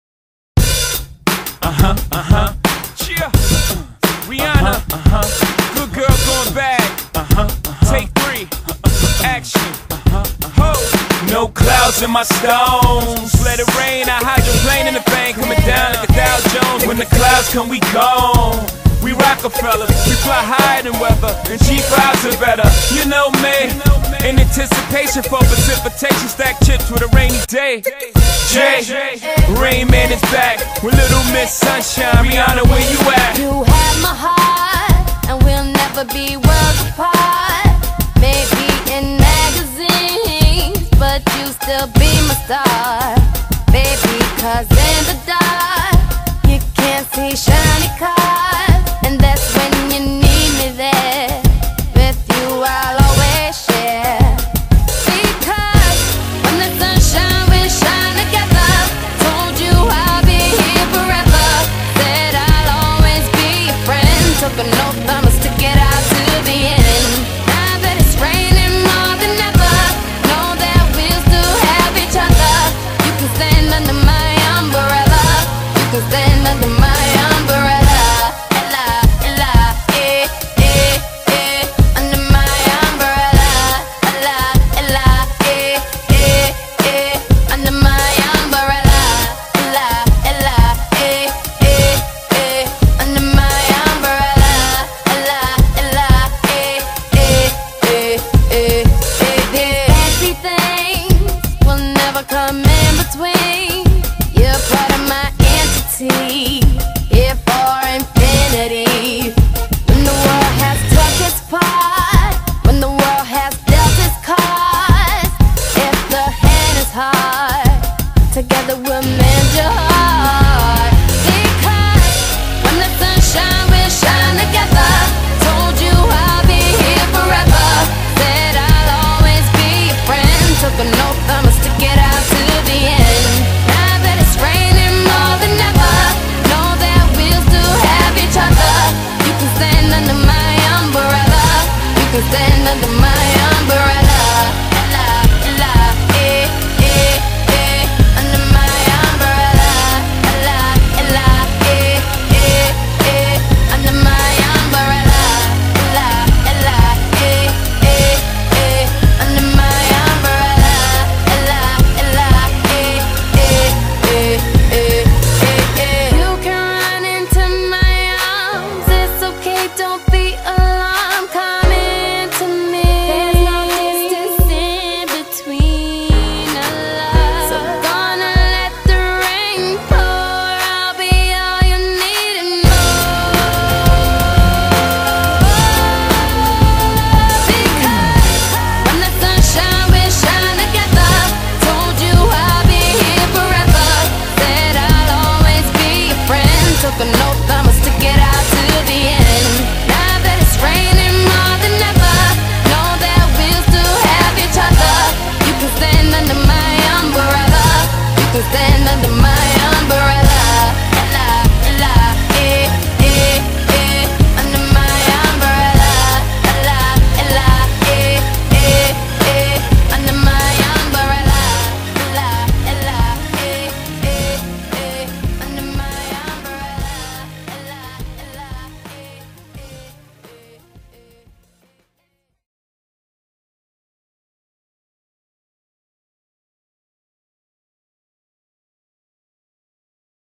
Umbrellas in Edinburgh
We went to Edinburgh for the Festival. It rained. We got wet jeans. I made a video of it.